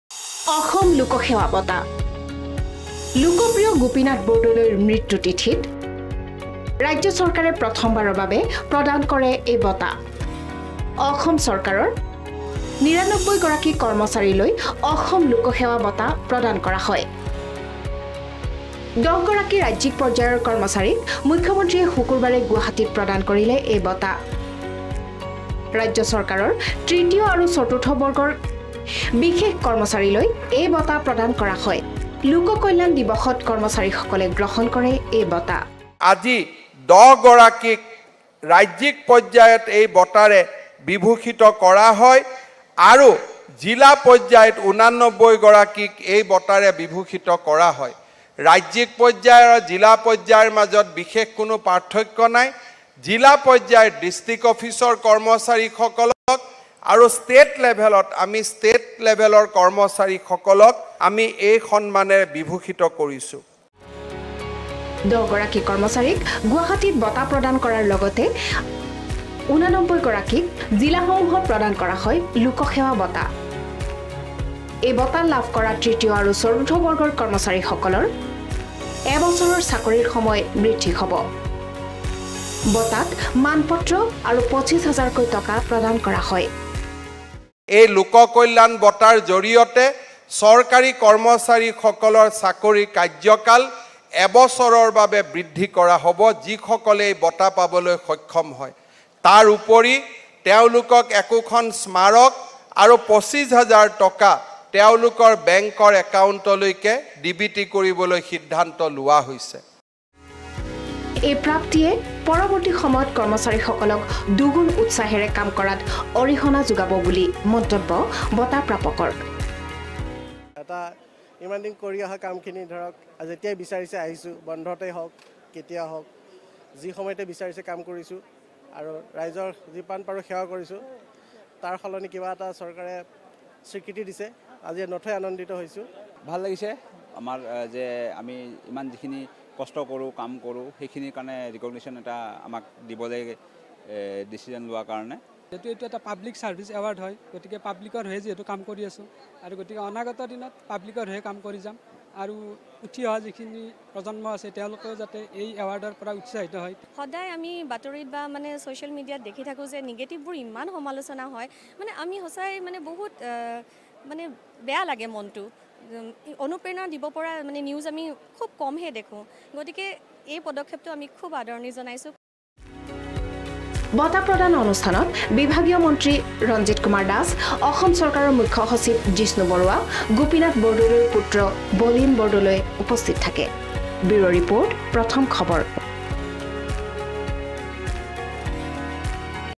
ৰাজ্য চৰকাৰৰ উদ্যোগত সোঁৱৰণ প্ৰথমগৰাকী মুখ্যমন্ত্ৰীৰ মৃত্যুতিথি
অসমৰ প্ৰথমগৰাকী মুখ্যমন্ত্ৰী লোকপ্ৰিয় গোপীনাথ বৰদলৈৰ স্মৃতি দিৱস ৰাজ্য চৰকাৰৰ উদ্যোগত পালন কৰা হয়। লোকপ্ৰিয়ৰ মৃত্যু তিথি উপলক্ষে আজি ৰাজ্য চৰকাৰৰ বহুকেইগৰাকী বিষয়ালৈ প্ৰদান কৰা হয় অসম লোকসেৱা বঁটা।